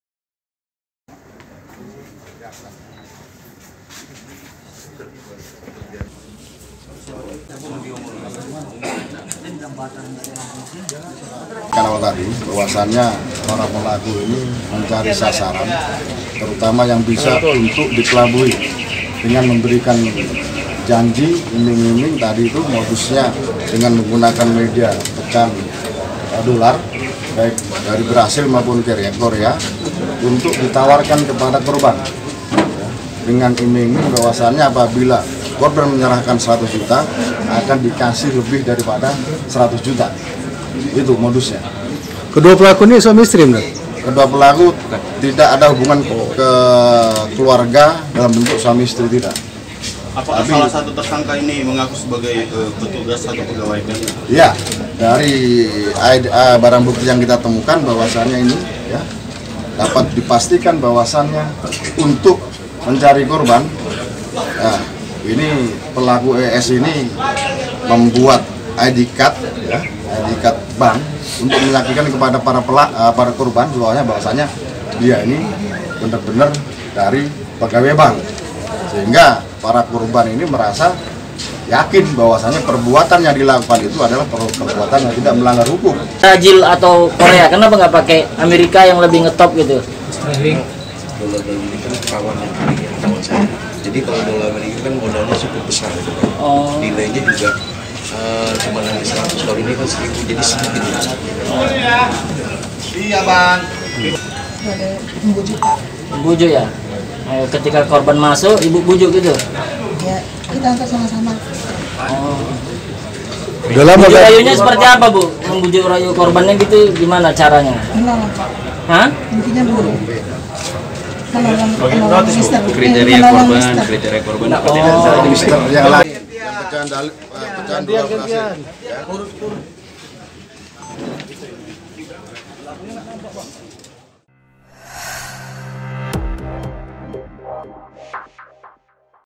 Perdaya Korban Tukarkan Uang Asing di Sejumlah Kota, 2 Tersangka Sindikat Penipuan Ditangkap Polisi
TRIBUN-MEDAN.com, KISARAN - Sat Reskrim Polres Asahan berhasil meringkus dua dari empat tersangka sindikat penipuan di Bandara Soekarno Hatta, Banten pada Senin (2/3/2020) sekitar pukul 15.00 WIB.

Kedua tersangka yang tertangkap yaitu Eri Setiawan warga Cikini, Jakarta dan Rita Sari, warga Metro Lampung, Bandar Lampung.

Kedua tersangka ini turut terlibat memperdaya korbannya, Rumondang warga Kisaran, Kabupaten Asahan.

Dalam kasus penipuan modus penukaran uang sebesar Rp 132 juta milik korban ke mata uang Real, Brazil.

Kedua tersangka berhasil tertangkap berkat penyelidikan petugas Unit Jatanras Polres Asahan berkoordinasi dengan Dit Reskrimum Polda Sumut dan Dit Reskrimum Polda Banten.